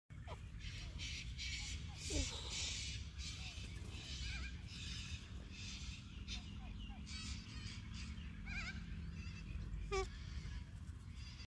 beautiful cockatoo 4
cockatoo is one of the beautiful birds with its vibrant color. This exotic bird is native to Australia. Generally, they are very friendly birds and stay with human.